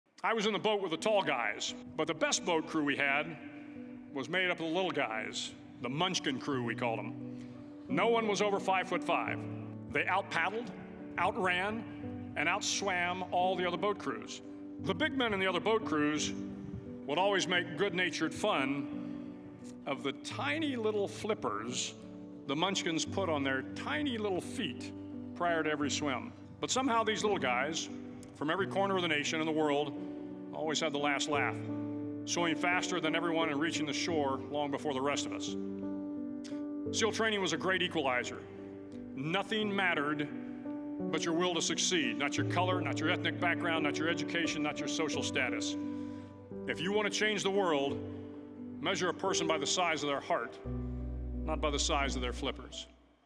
Admiral McRaven and 'Judging a Book By Its Cover'
Source: University of Texas at Austin 2014 Commencement Address - Admiral William H. McRaven